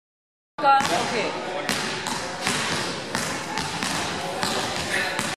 Rollen auf dem Barren mit Hilfestellung und Matte (2)
Die Rollbewegung am Barren bietet für viele neue Bewegungs- und Körpererfahrungen. Sie wird mit Hilfe einer methodischen Übungsreihe am Boden, auf den kleinen Kästen bis hin zum Barren erlernt.